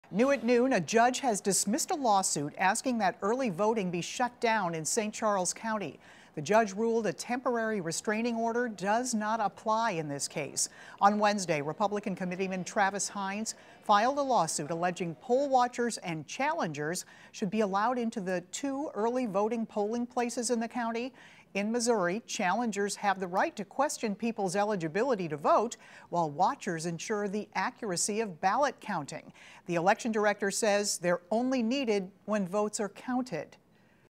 St. Charles County election lawsuit dismissed by judge
A judge has dismissed a lawsuit asking that early voting be shut down in St. Charles County. They ruled a temporary restraining order does not apply in this case.